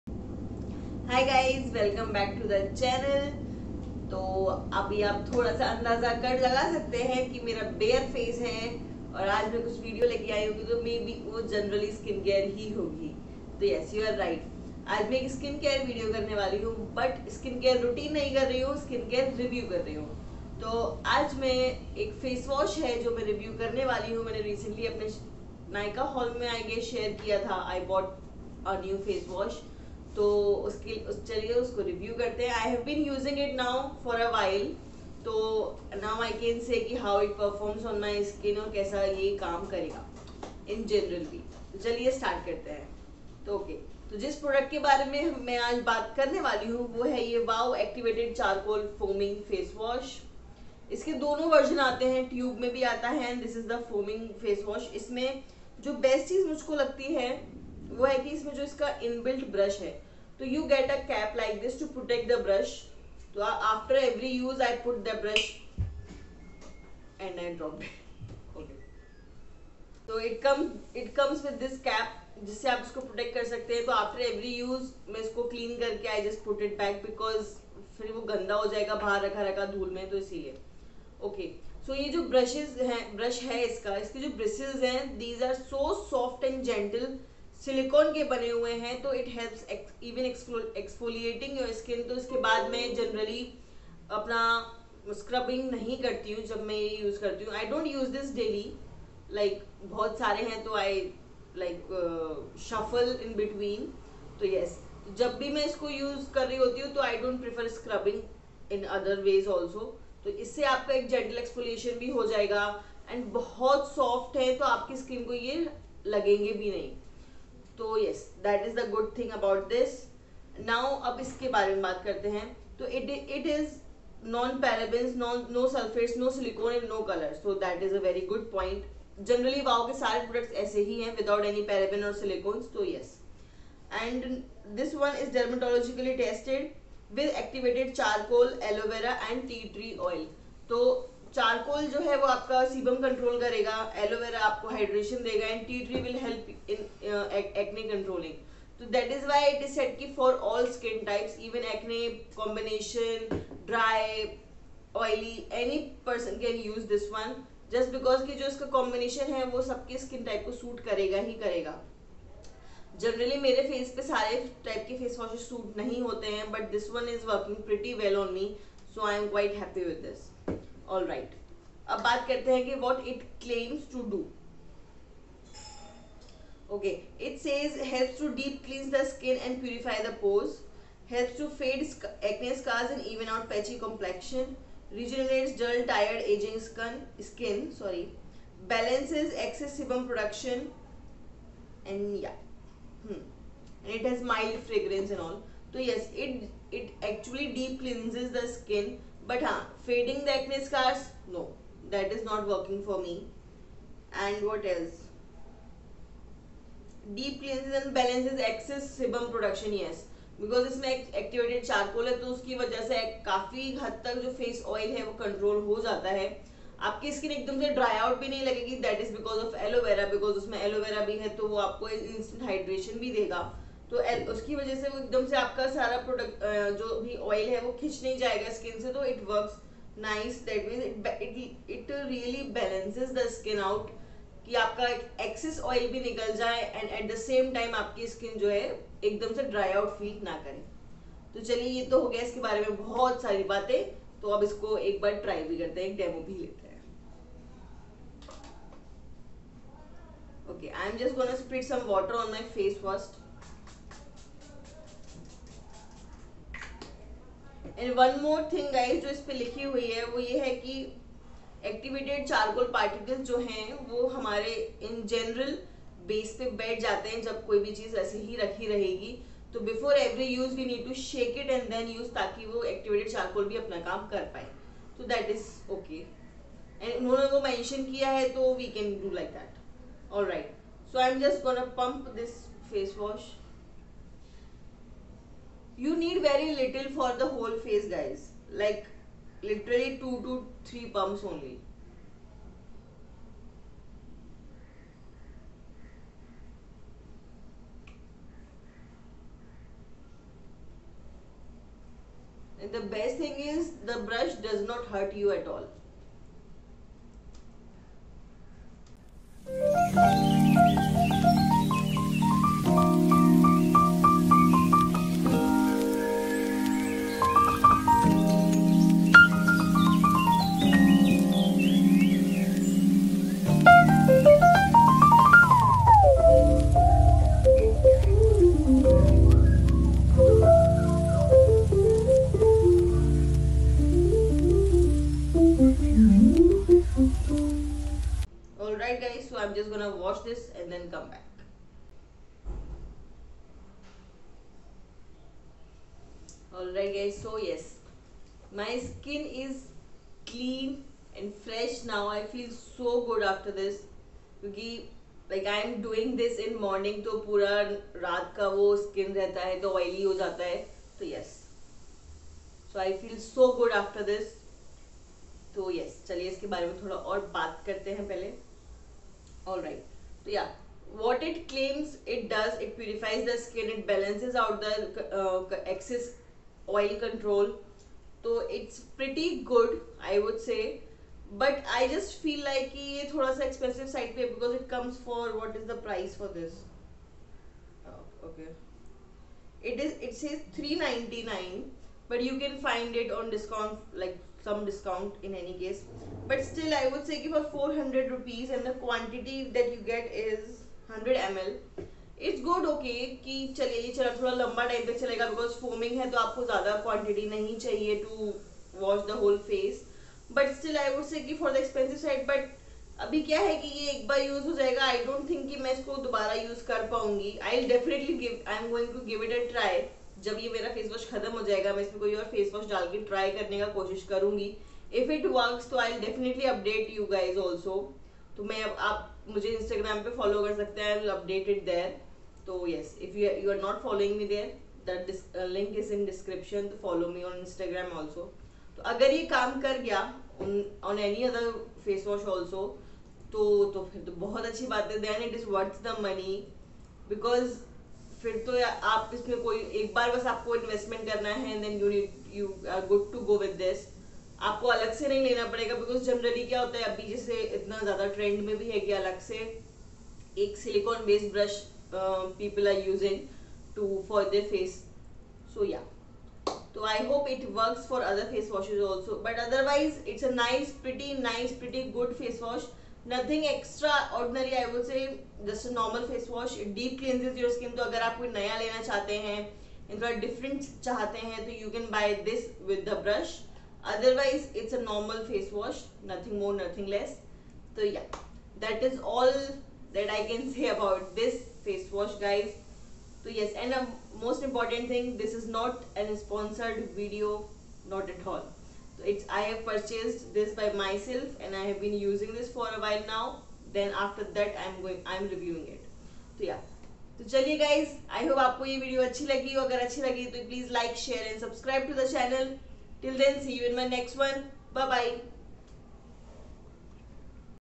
WOW ACTIVATED CHARCOAL FOAMING FACEWASH |REVIEW DAY |*NOT SPONSORED |SKINCARE |beautybaewith_nim
Hi guys
This facewash was recently launched by WOW Skin Science And I have been using it for a long now.

The lighting in this video is not good because I shoot in normal daylight and this day it was cloudy.